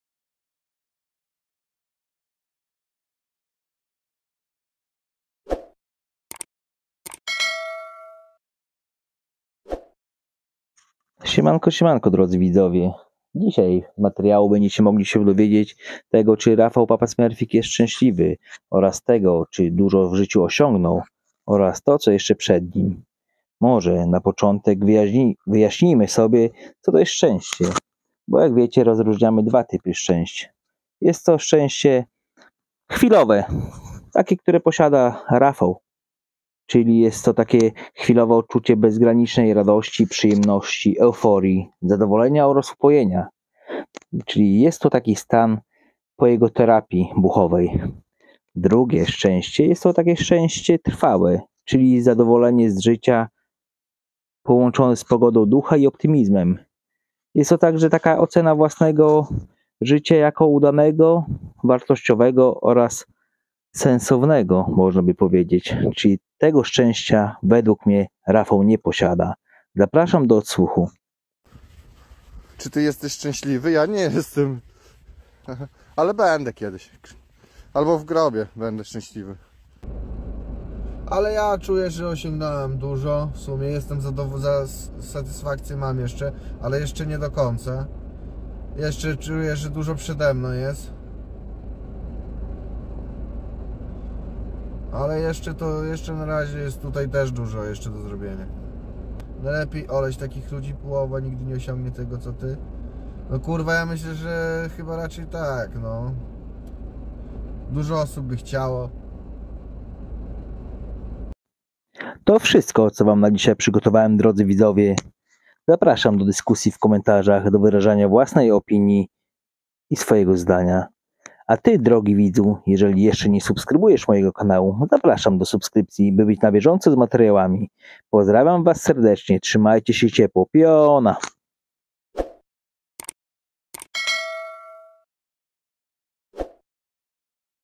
Papa Smerfik CZY JEST RADOSNY???
Rafał papasmerfik reżyser serialu o bezdomni mówi o swoim happy .